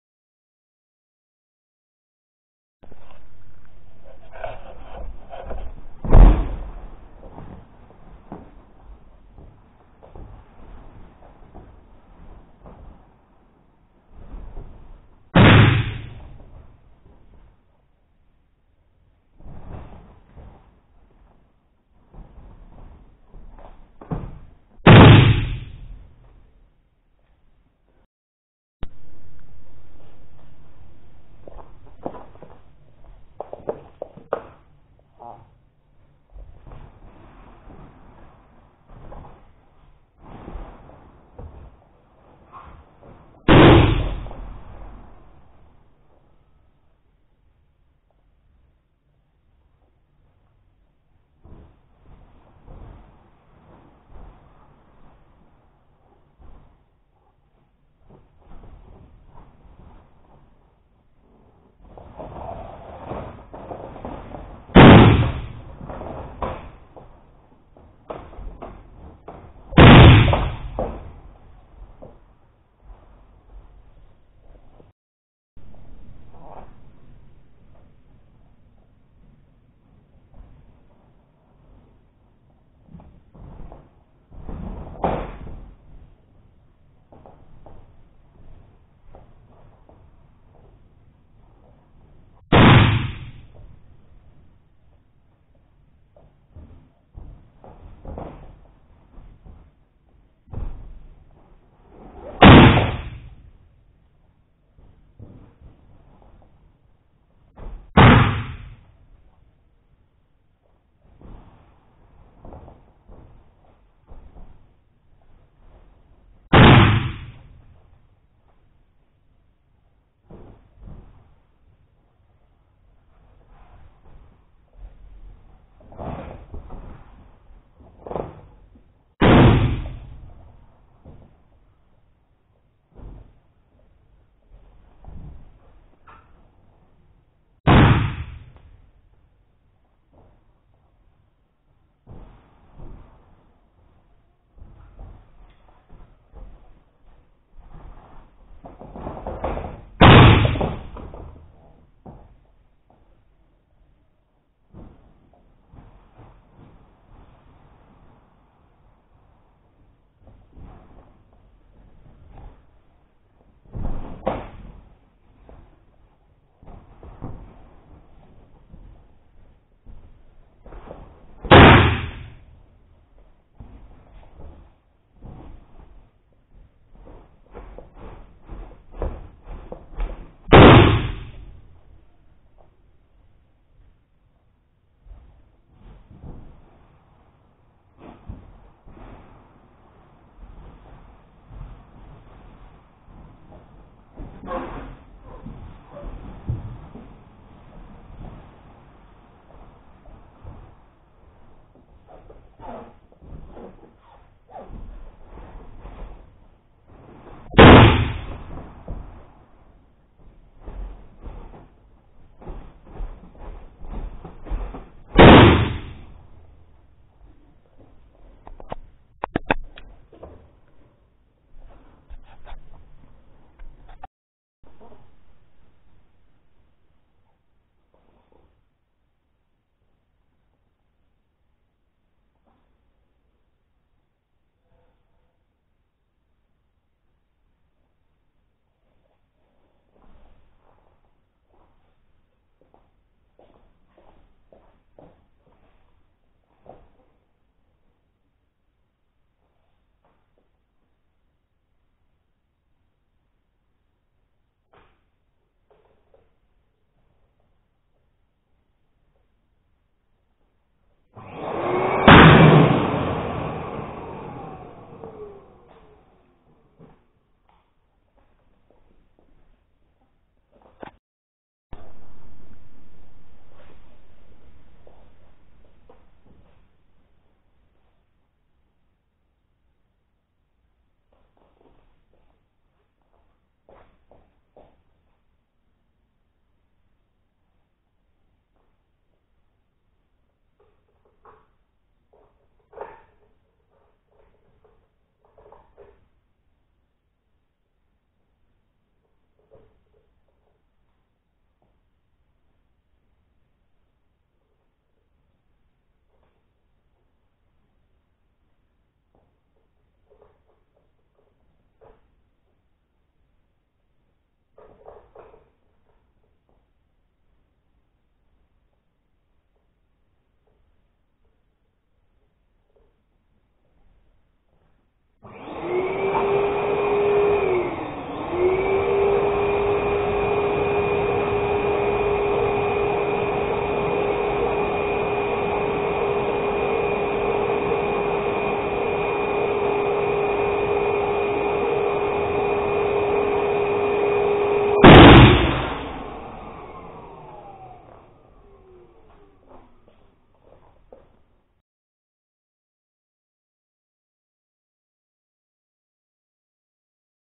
Session 1: POP Hands, Foot, Sit, Fail and Siamese Balloons
En este video os traigo lo que he llamado ( siamese balloons ) que son dos globos que han sido pegados de las bocas. Mas un poco de variedad.